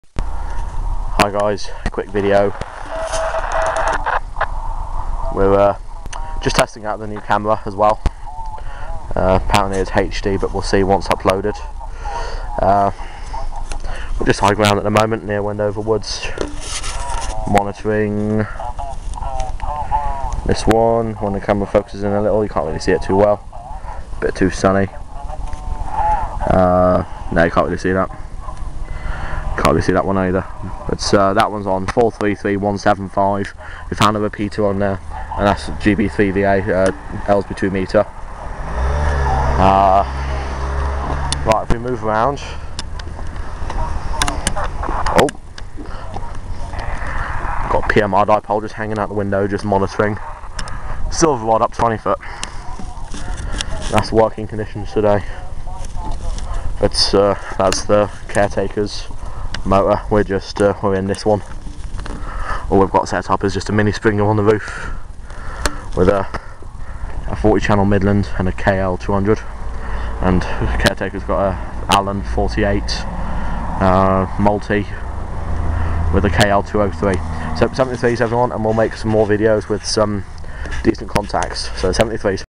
CB Radio: Sunday Afternoon DX - 19 / 02 / 2012 - Location / Setup Information
Camera quality a bit iffy, but never mind